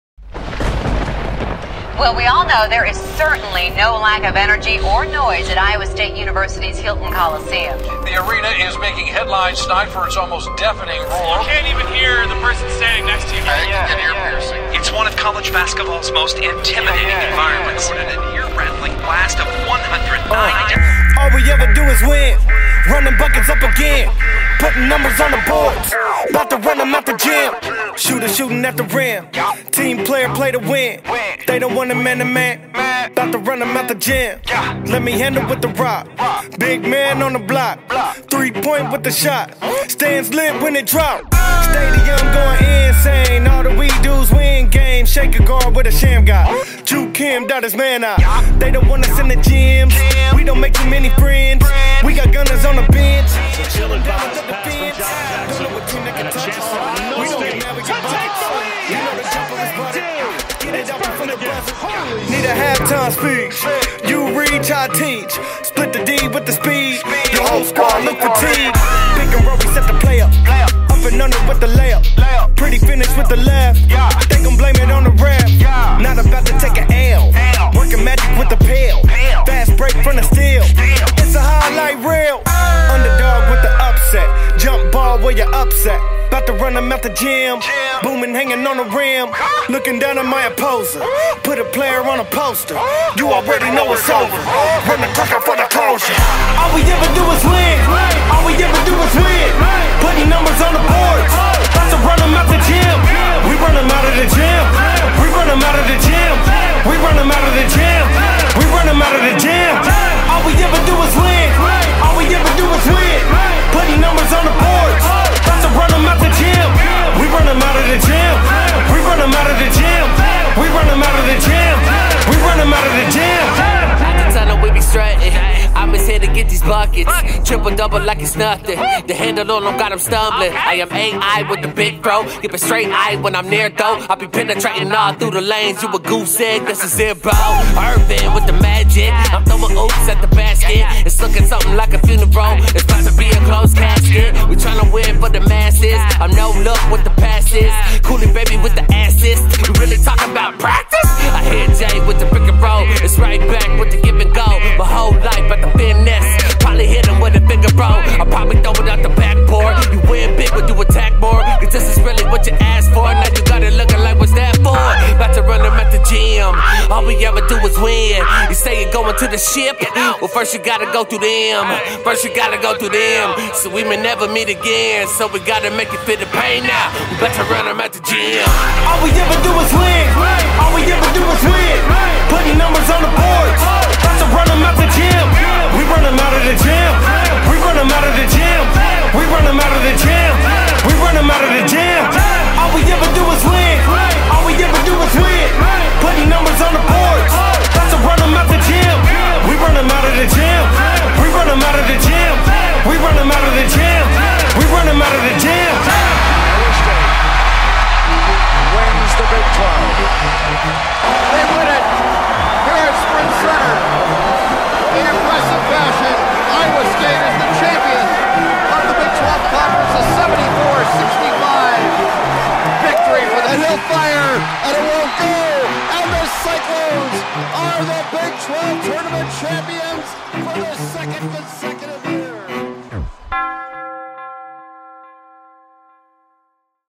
Iowa State Big XII Tournament Hype Video
Iowa State will go for it's 3rd Big XII Tournament title in four years. Music credit Jake McDonough.
Video credit: ESPN, Cyclones.tv